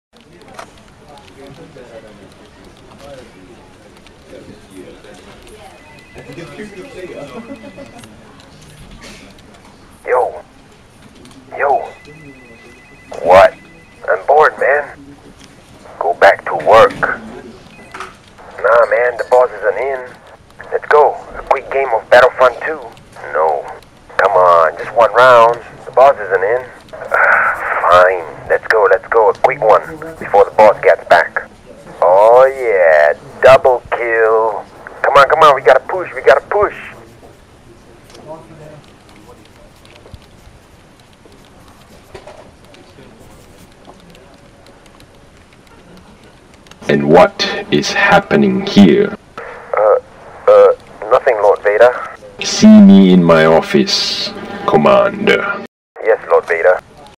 Star Wars : Office Fan Film (4K)
First few attempts at 3D Animations on Blender, still trying to improve it. Combining two of my favourite shows together.

I do not own star wars or band of brothers. This is completely for fun.